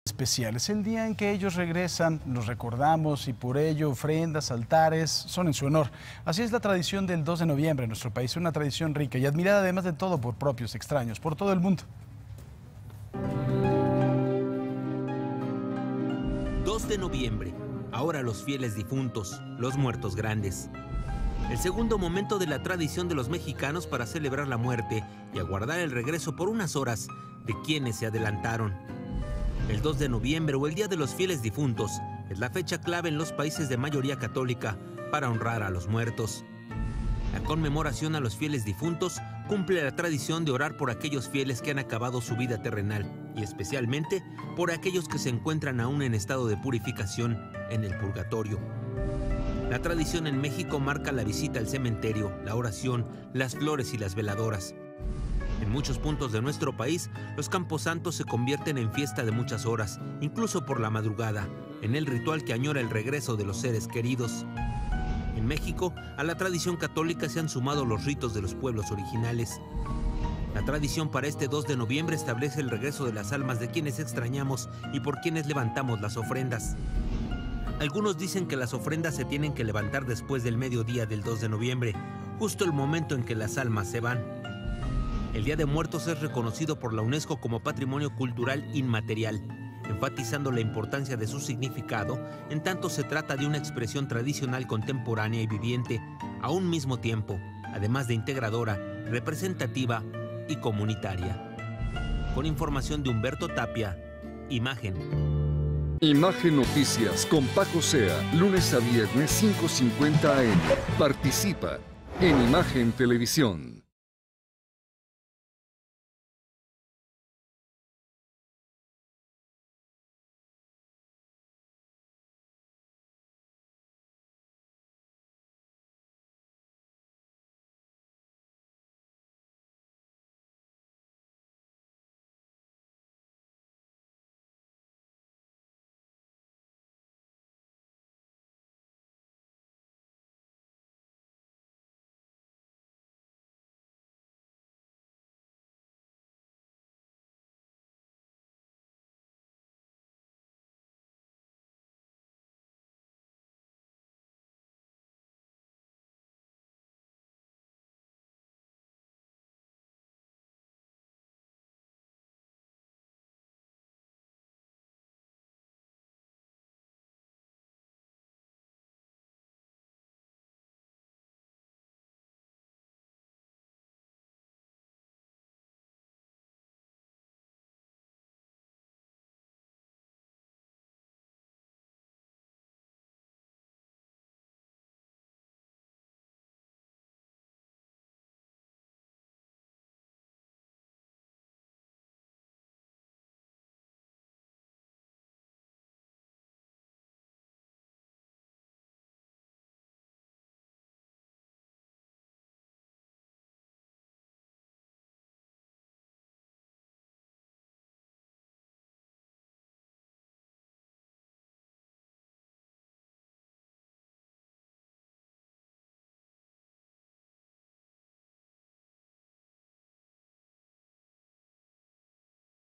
Así celebramos a la muerte el 2 de noviembre en México | Noticias con Francisco Zea
Durante el día de los muertos los mexicanos tenemos muchas tradiciones, la tradición para este 2 de noviembre establece la vuelta de las almas de los familiares ya fallecidos.